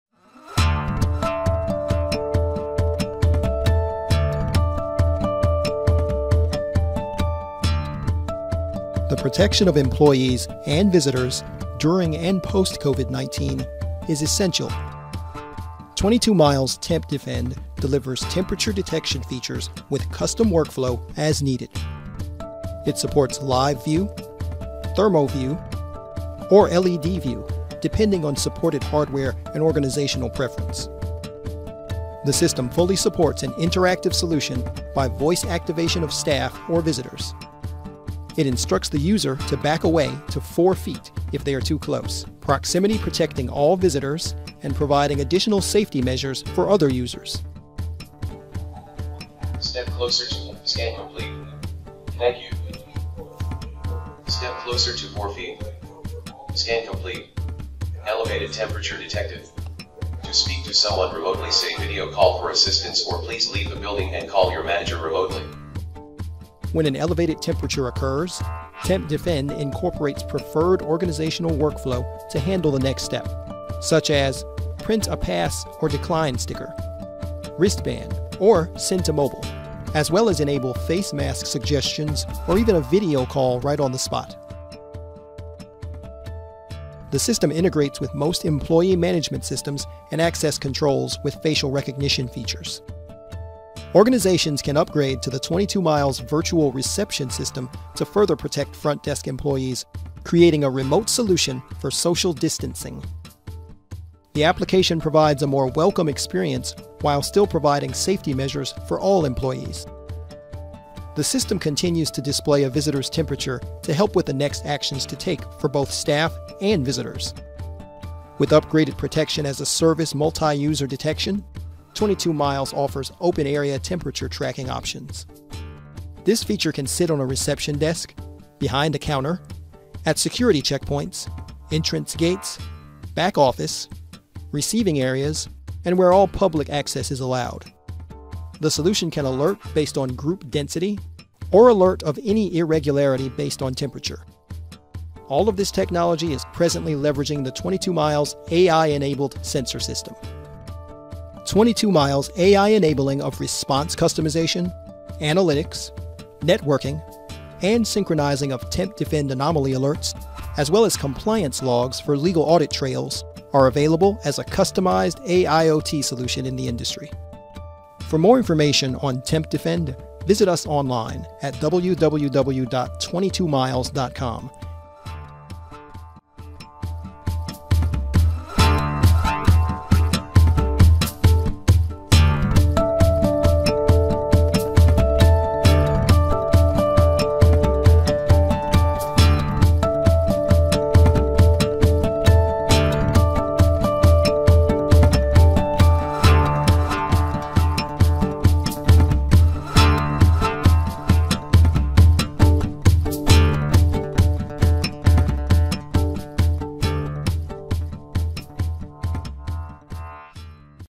Temperature Detection Software - Thermal Screening Kiosks - TempDefend by 22MILES
Protecting yourself, staff, visitors, and patrons from the spread of contagious diseases begins with identifying, quarantining, and treating infected individuals. 22MILES offers TempDefend, a thermal screening software solution that provides the latest thermal sensor technology to organizations seeking to ensure safety in the age of viral pandemics.

TempDefend Protection as a ServiceTM  packages are now available from 22MILES and can be customized to the needs of your organization. TempDefend is compatible with your own sensor hardware or 22MILES suggested/provided sensors.

For a thermal sensing kiosk, temperature screening software, or thermo-detection solution consultation/quote,